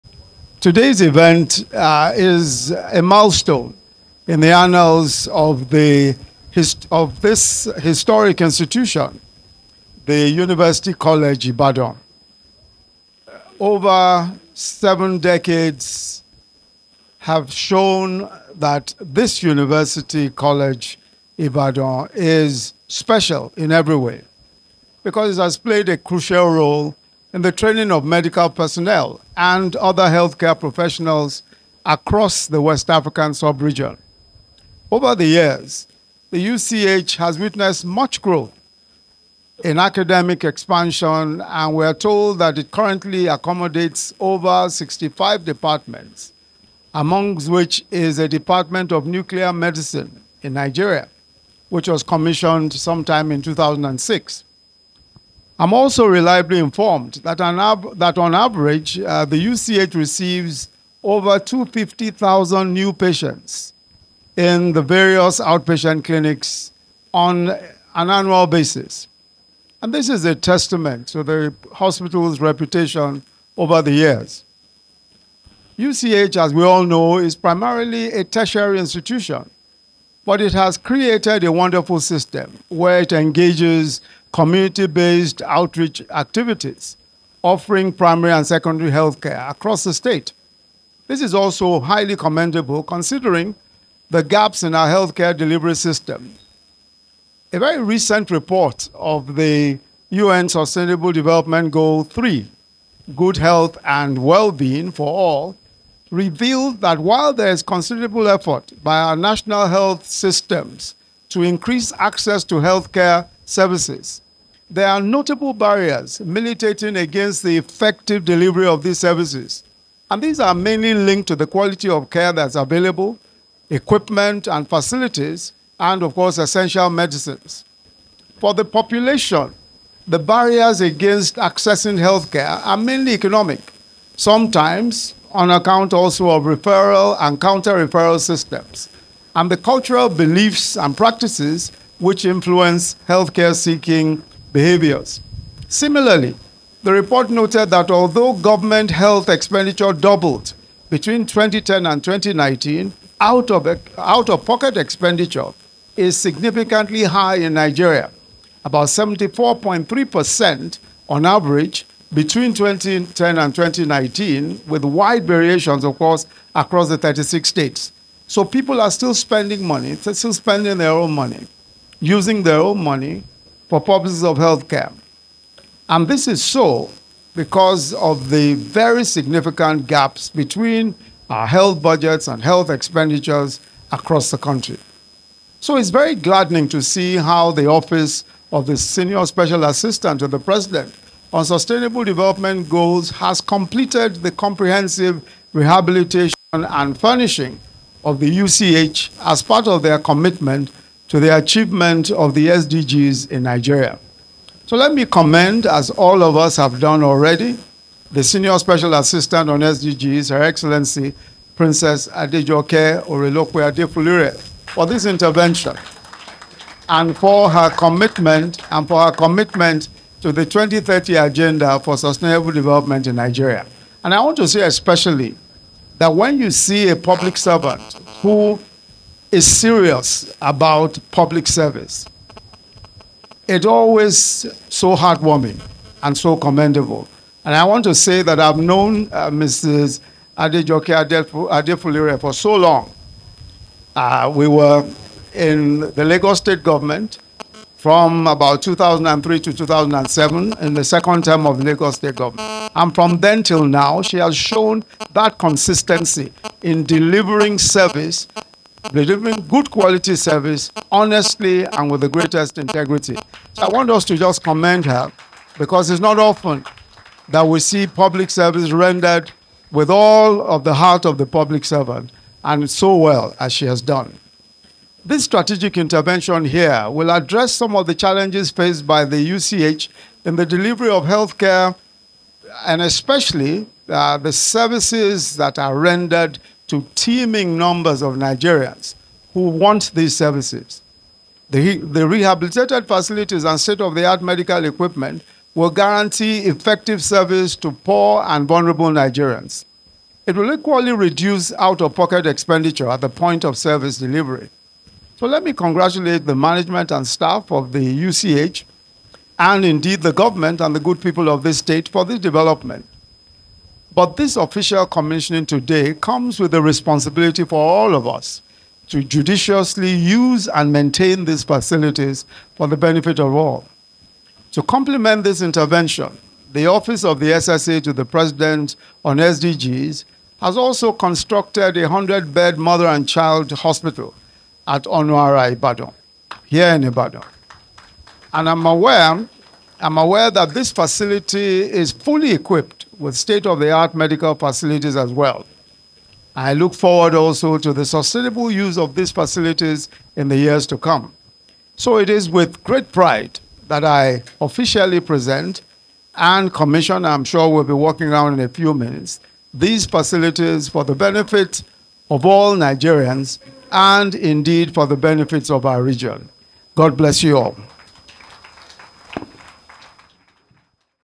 Barriers To Accessing Healthcare Are Mainly Economic, Also Based On Cultural Beliefs And Practices
Remarks at the Commissioning Of Rehabilitated Parts Of The University College Hospital, Ibadan By The OSSAP - SDGs

"The comprehensive rehabilitation and furnishing of the hospital will guarantee effective service delivery to poor and vulnerable Nigerians. It will equally reduce out-of-pocket expenditure at the point of service delivery.”

Prof. Osinbajo noted that while for over 7 decades UCH "has played a crucial role in the training of medical personnel and other healthcare professionals across the West African sub-region”, the need to upgrade facilities had been a challenge.

He said, “this strategic intervention will address some of the challenges faced by the University College Hospital in the delivery of healthcare services for the teeming Nigerians demanding these services.”

The VP added that the intervention is “part of the government’s commitment to the achievement of the SDGs in Nigeria,” and commended the Senior Special Assistant to the President on SDGs, Princess Adejoke Orelope-Adefulire “for this intervention and for her commitment to the 2030 Agenda for sustainable development in Nigeria.”

Oluyemi Oluleke Osinbajo is a Nigerian Lawyer and Politician who is the current Vice President of Nigeria, in office since 29 May 2015.